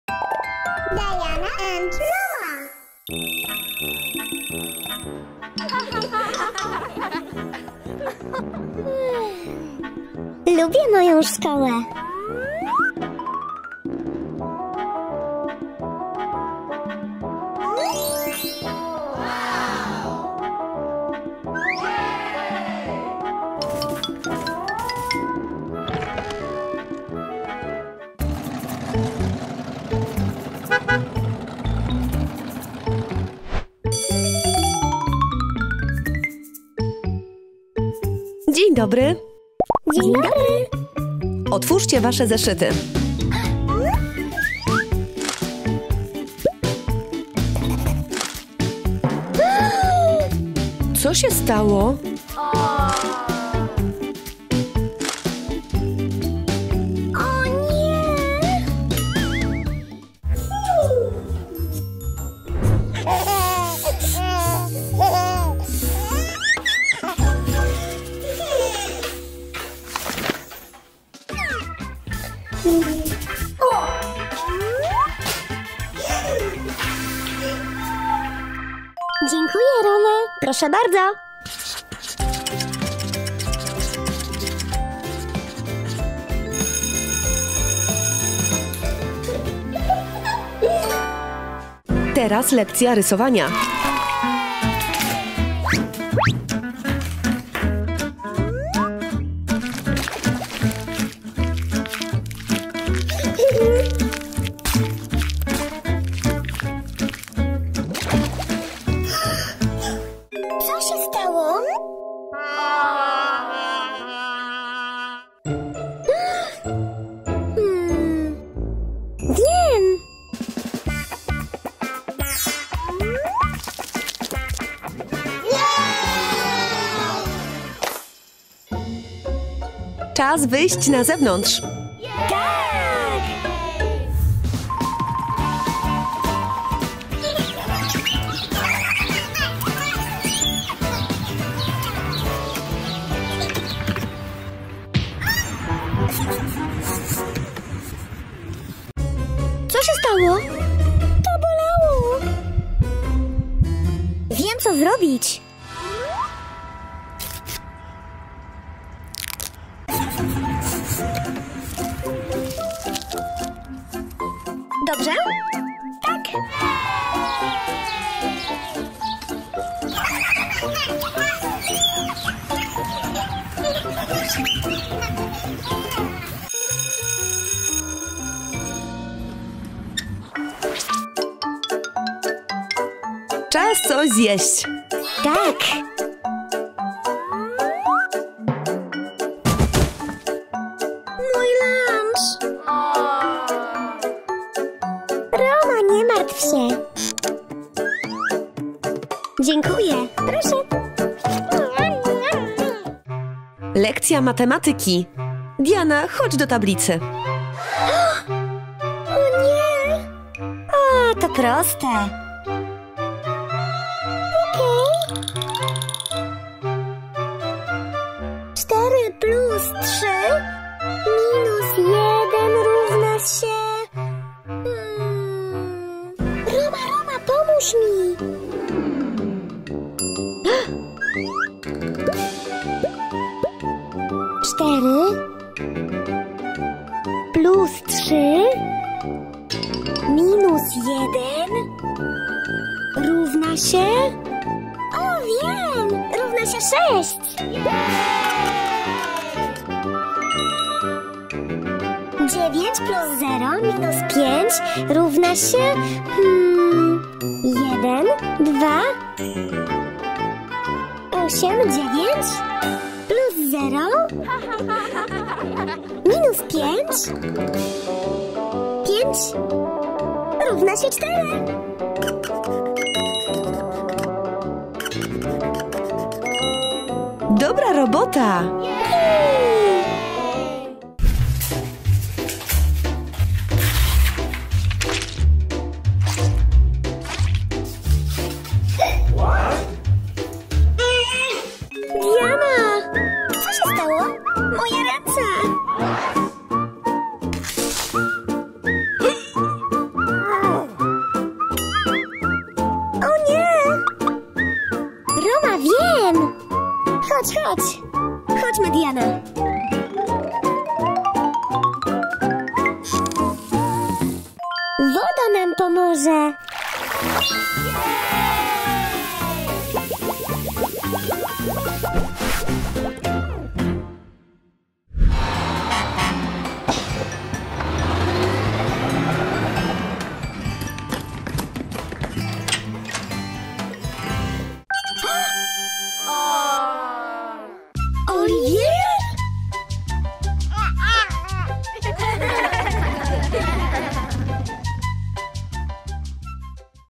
Diana i Roma pokazują jak pomagać sobie nawzajem w szkole
Diana i Roma dzielą się wskazówkami jak pomagać sobie w szkole